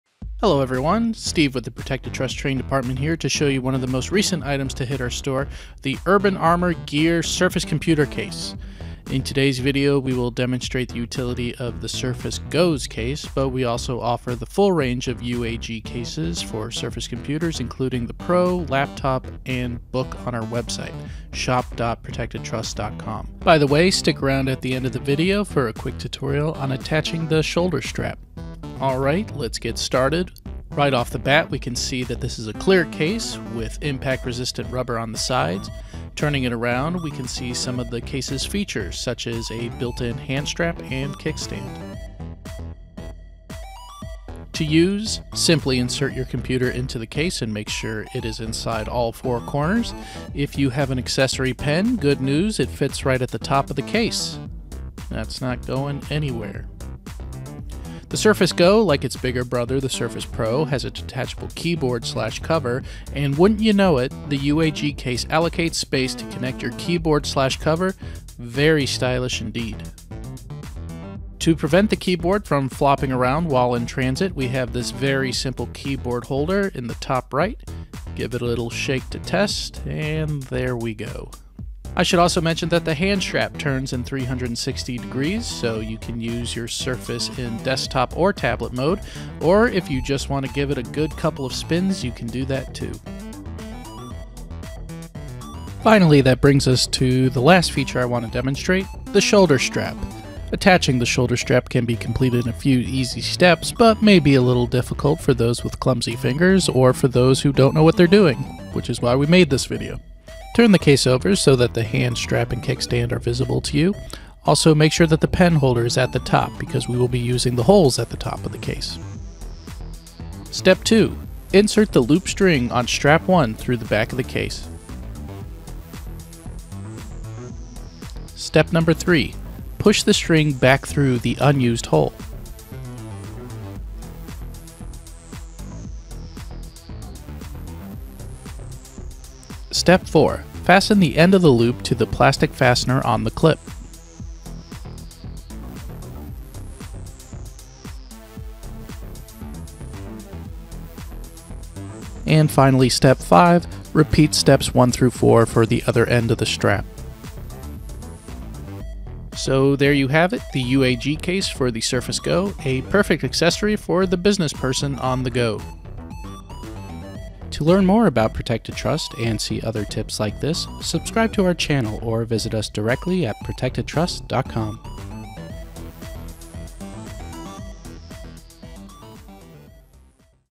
Hands On with UAG Rugged Case for Surface Computers
Don't buy a cheap and ineffective case to protect your expensive computer.

In this hands-on review, Steve demonstrates a few features on the UAG case for Microsoft Surface Go computers available